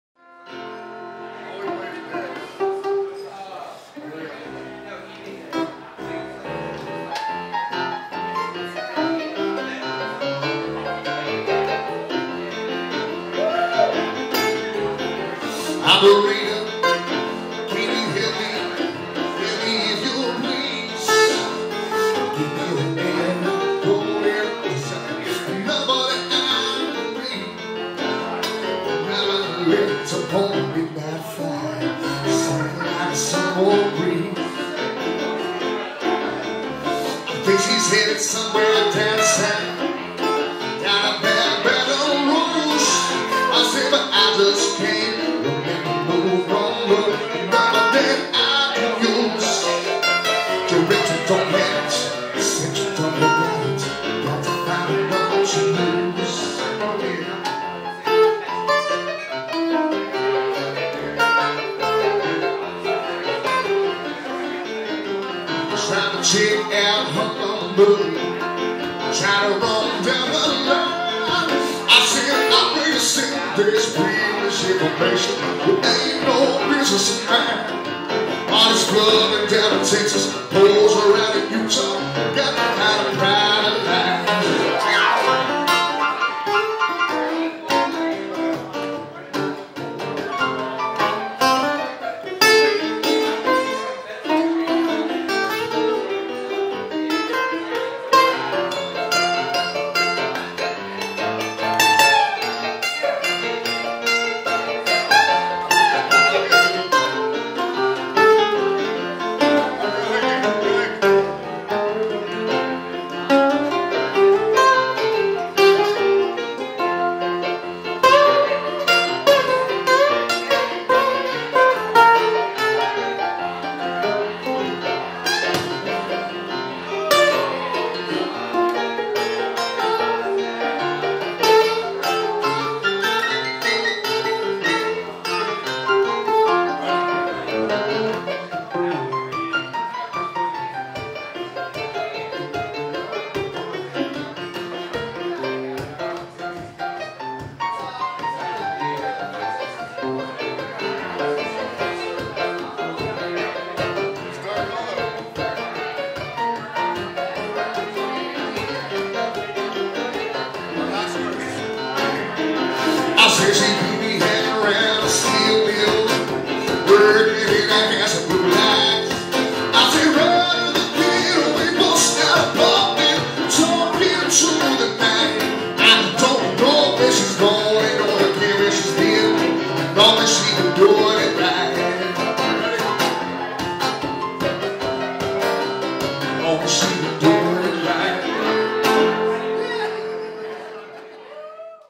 Chris Workman & Craig Miller @ Bullwinkles- 5
Chris Workman & Craig Miller play during open mic night at Bullwinkles in Cumberland, MD